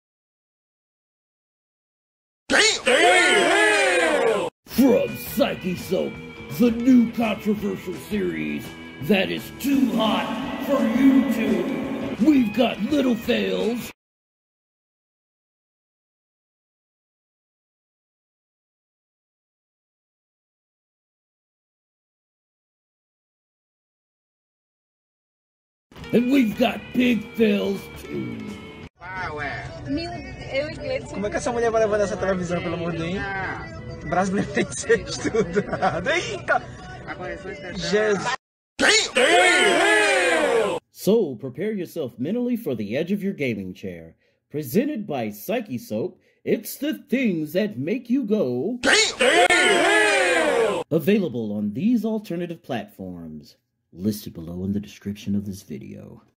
Things That Make You Go Daaamn (preview 28 - indescribablehorror)
AVAILABLE ONLY ON MY ALT-TECHS! From the sea of randomness comes indescribable horror. People are afraid of what they don't understand. Contained within are some dark web goodies and very very horrifying situations.

CHECK ME OUT ON DISCORD AND TELEGRAM!!!